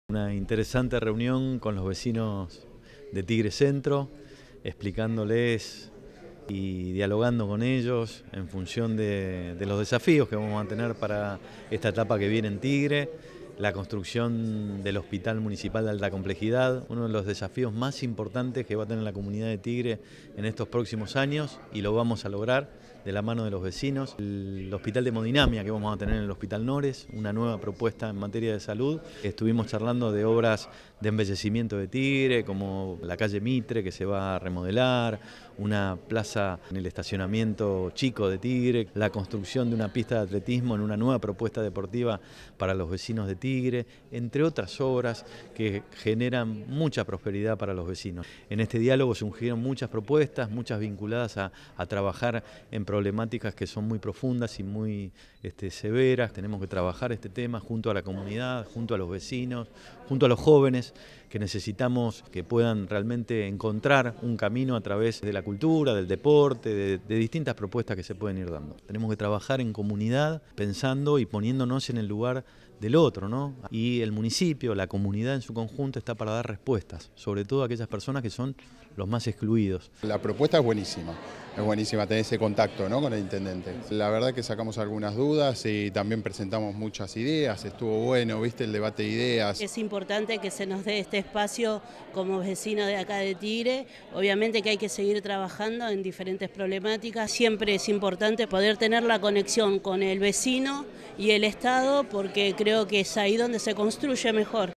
Consejo consultivo de Tigre centro
Julio Zamora en Tigre centro: “El Hospital de Alta Complejidad va a ser el desafío más importante en años y lo vamos a construir de la mano de la comunidad”

Lo aseguró el intendente en la reunión junto al Consejo Consultivo de la localidad, donde analizó generar propuestas en materia de adicciones y un reordenamiento del tránsito en el casco urbano del partido. Además, comunicó que en el Hospital Valentín Nores funcionará un campo de hemodinamia y que “la salud será el eje central para la gestión en los años que vienen”.